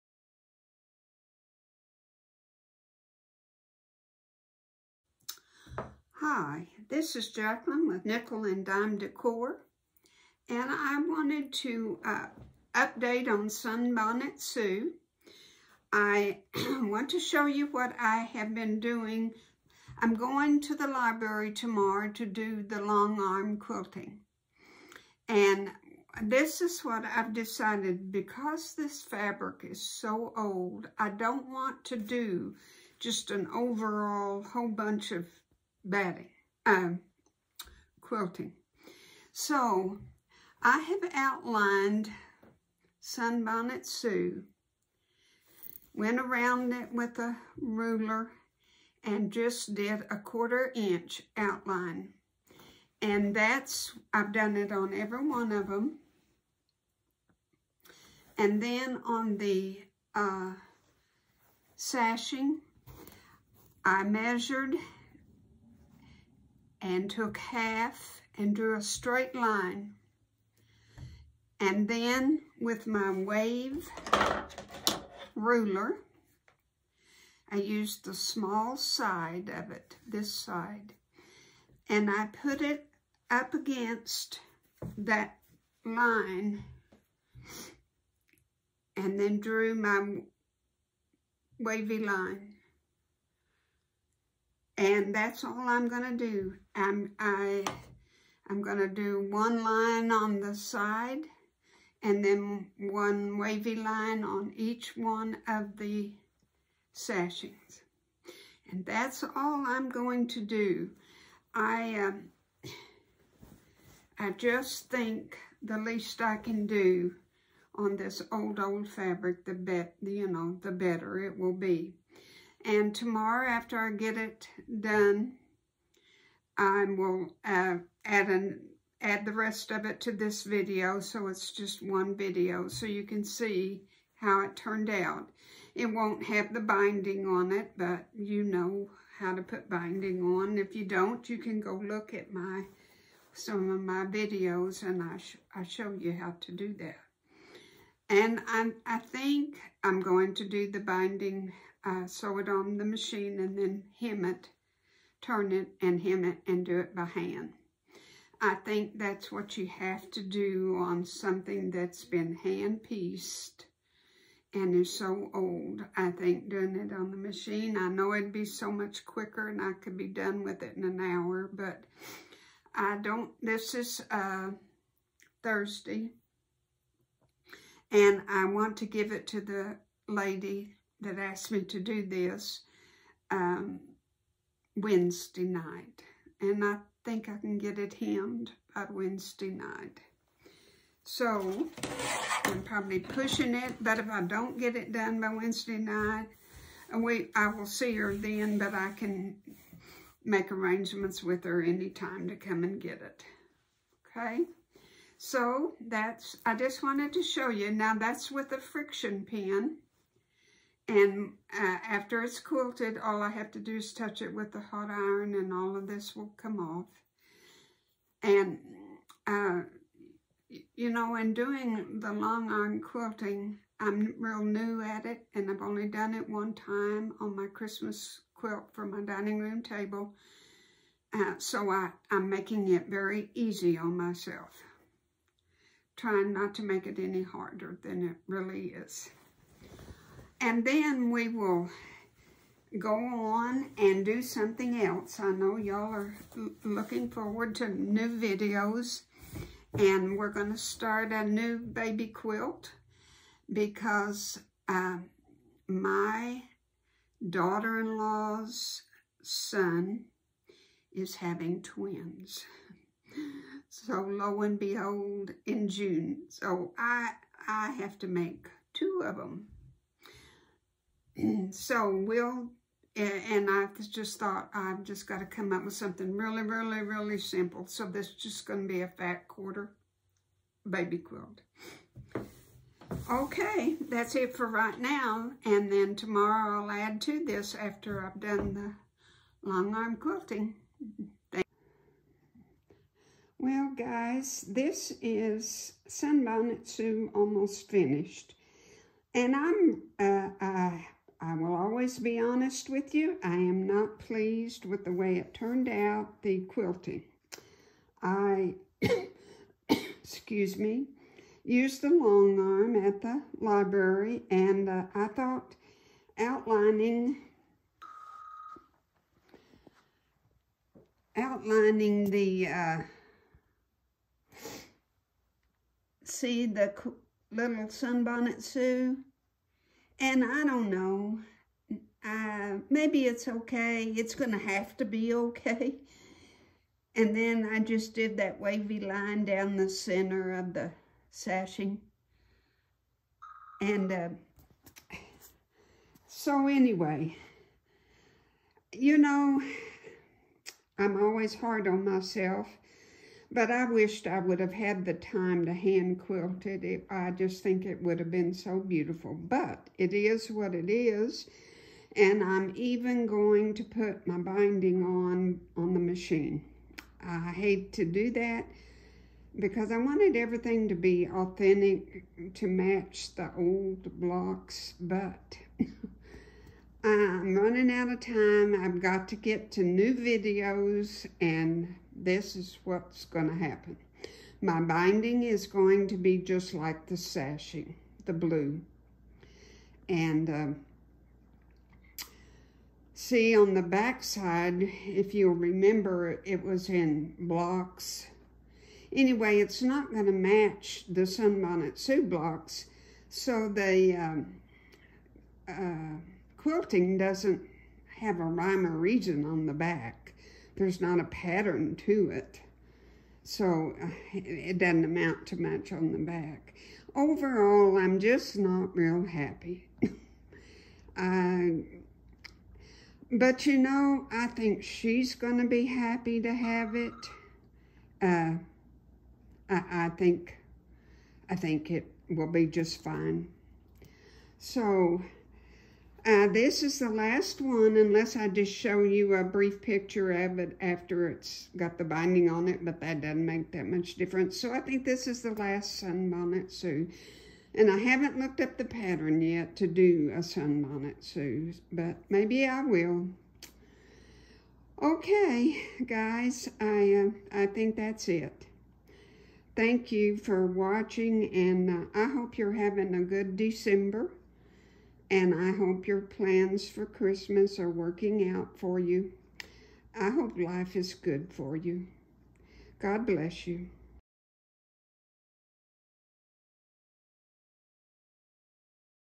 Sun Bonnet Sue Finished
Sun Bonnet Sue Quilt finished. Machine quilted outlines on sun bonnet sue and wavy lines on sashing. Simple quilting on an old block quilt. nikkelanddimedecor free tutorial for you to watch. Beginner quilting for beginners. Sun Bonnet Sue pattern is one of the oldest patterns there is and is often made with feed sacks. I think these were made just that way and hand pieced. My customer found the blocks and they were her mothers.